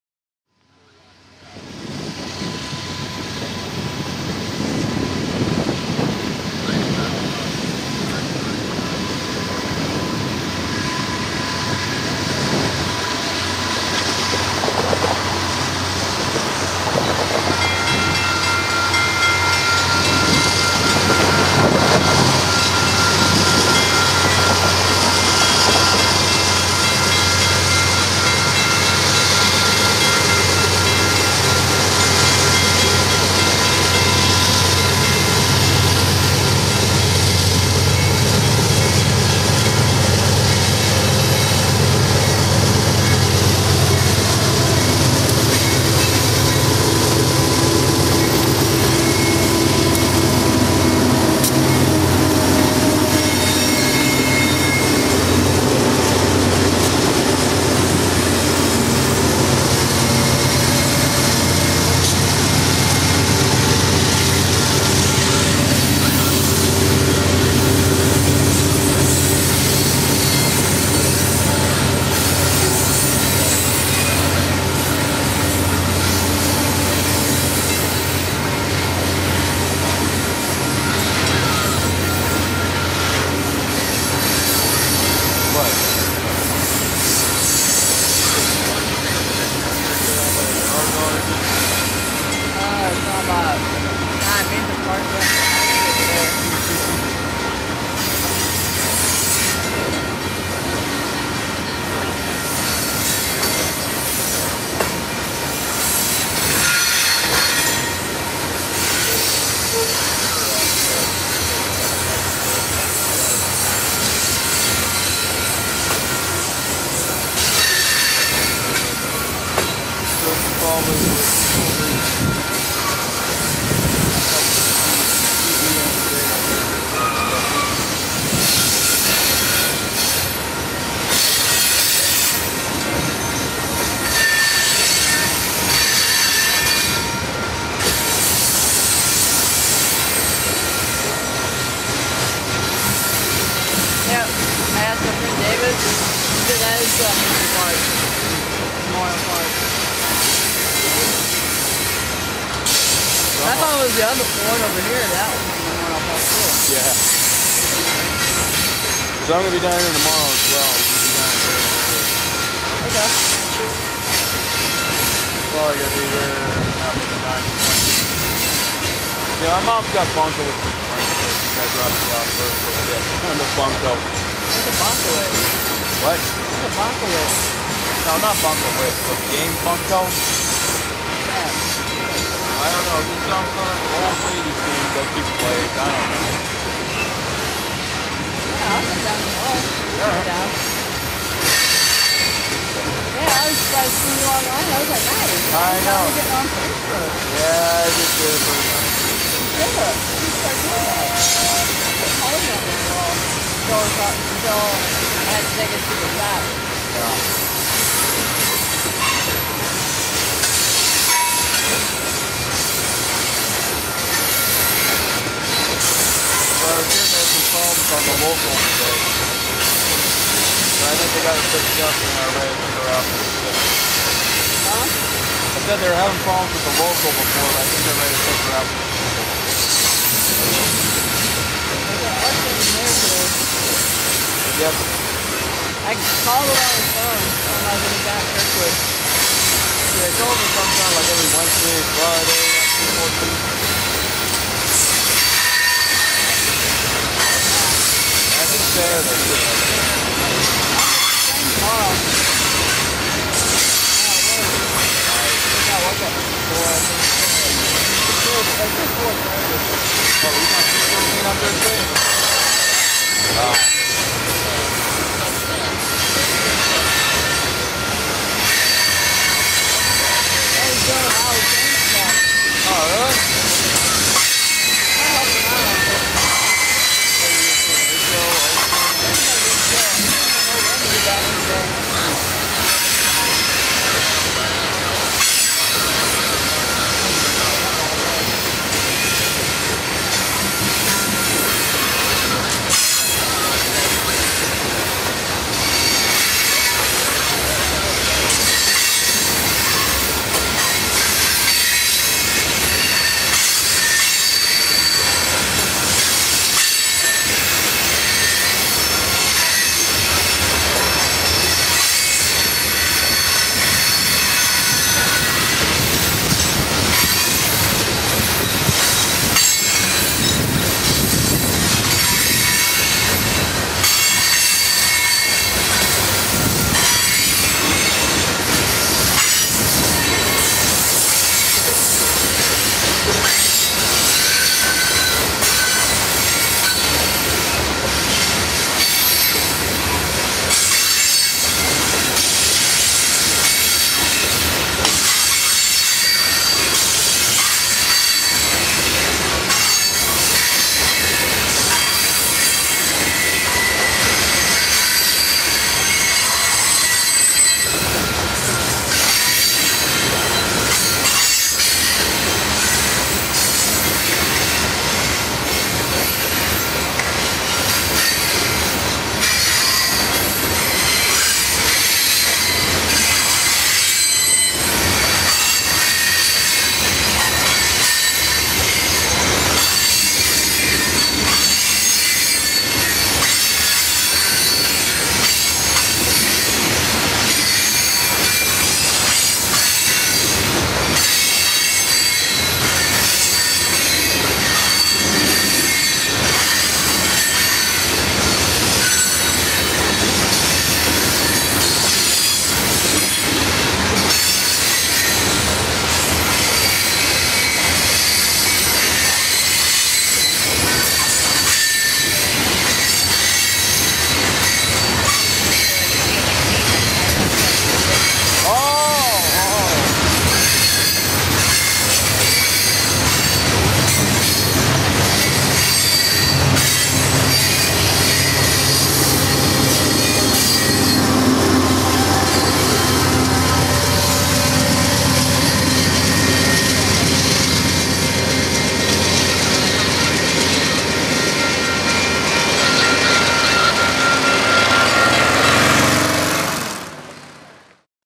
7/7/2010 UP 6836 Leads The CETJO9 Eastbound On 1
Switching to two at Webster